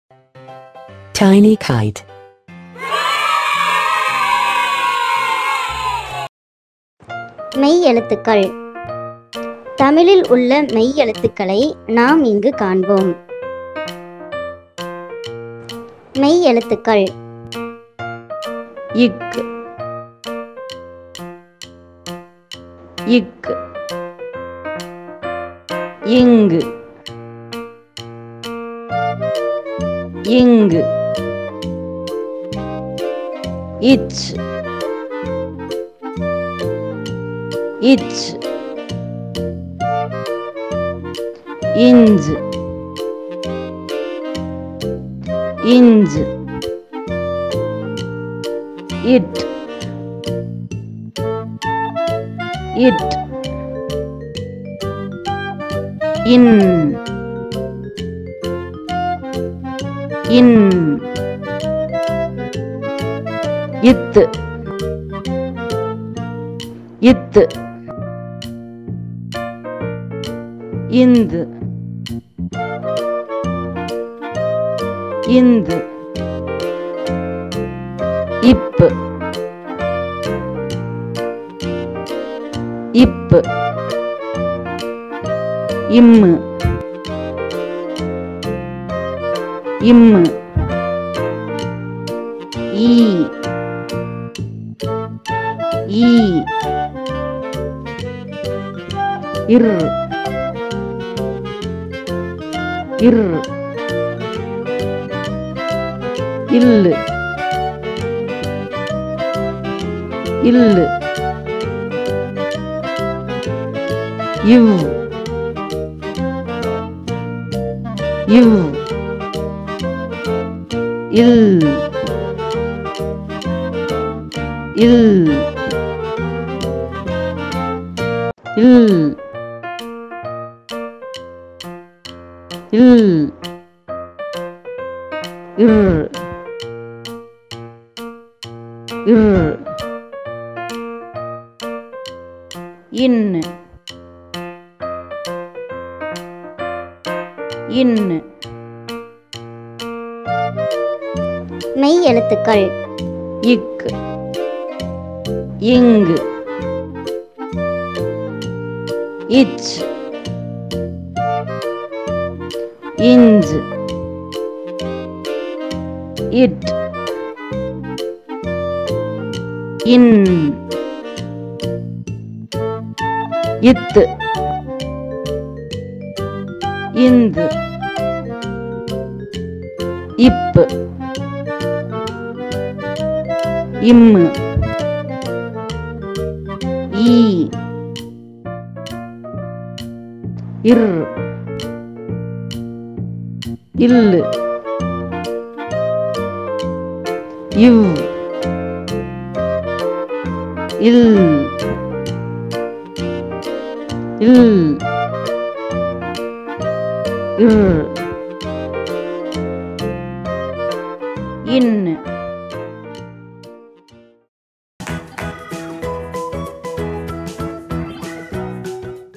எளிய முறையில் மெய்யெழுத்துக்கள் கற்றல் | Easy to learn Mei Ezhuthukkal in tamil | tiny kite
குழந்தைகளுக்கான எளிய முறையில் தமிழ் கற்றல் காணொளி. இந்த காணொளியில் குழந்தைகள் மெய்யெழுத்தை பற்றி பற்றி அறியலாம் .
Easy to learn Mei Ezhuthukkal in tamil for kids.